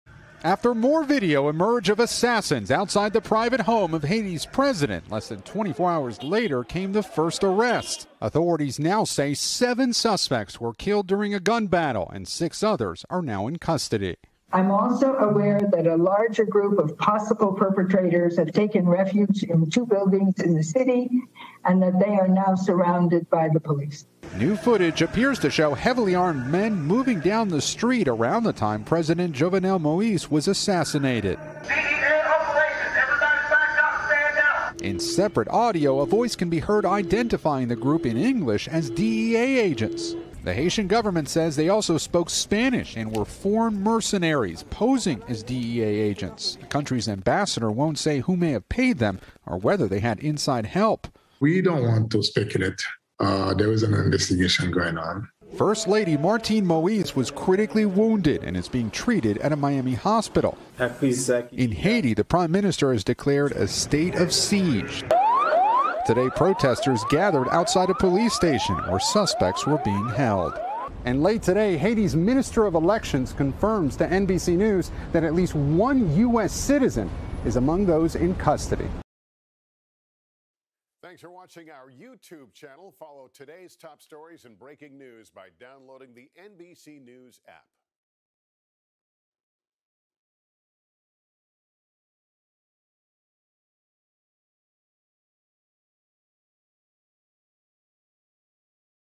New Details And Arrests In Assassination of President Jovenel Moïse
Less than 24 hours after the assassination of Haitian President Jovenel Moïse, Haitian authorities say seven suspects were killed during a gun battle and six others are now in custody.» Subscribe to NBC News